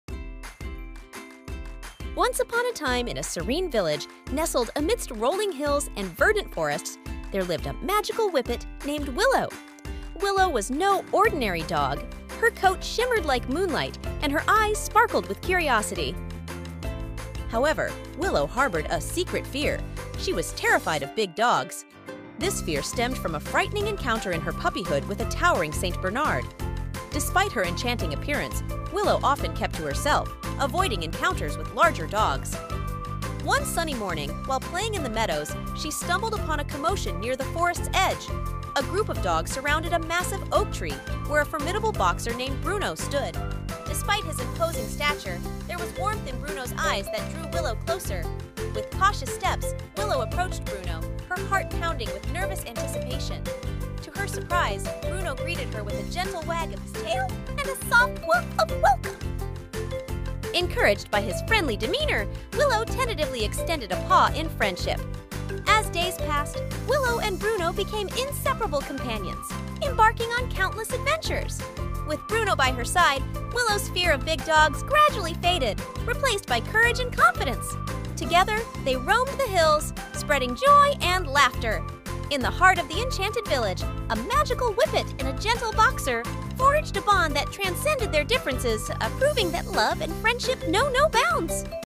Willow the Magical Whippet
A short story about Willow, a magical whippet dog.